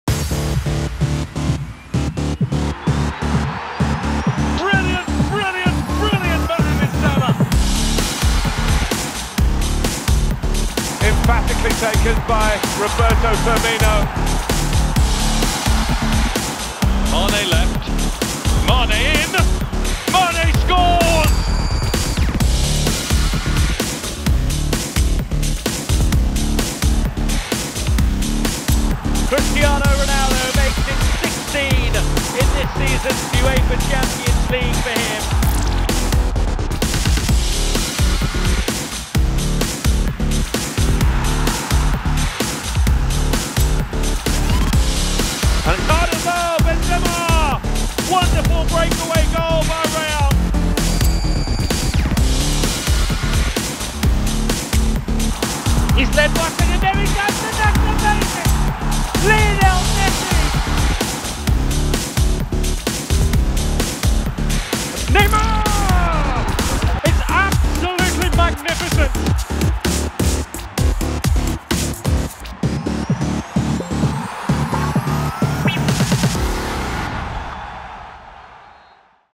SALAH, FIRMINO, MANÉ: Liverpool trio make history
Liverpool's Salah, Firmino and Mané have stolen the striking headlines in this season's Champions League, scoring a record 29 goals between them to set a new single-season competition standard for attacking trios.

29 goals: Salah, Firmino and Mané - 2017/18
28 goals: Cristiano Ronaldo, Benzema and Bale - 2013/14
27 goals: Messi, Neymar and Suárez - 2014/15